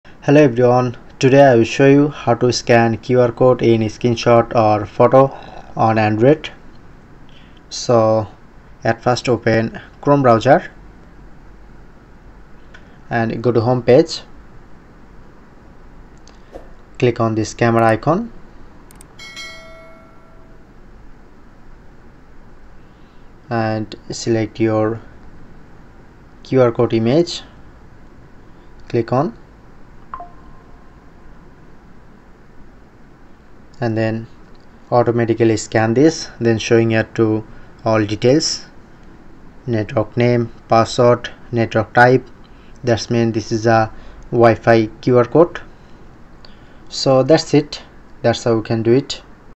How To Scan QR Code In Screenshot or Photo On ANY Android 2025 [Quick & Easy]
Learn how to effortlessly scan a QR code from a screenshot with our step-by-step guide. Understand different methods and tips for easy scanning.

Video Parts:
0:00 Info & Intro
0:10 Open Chrome Browser
0:47 Ending & Outro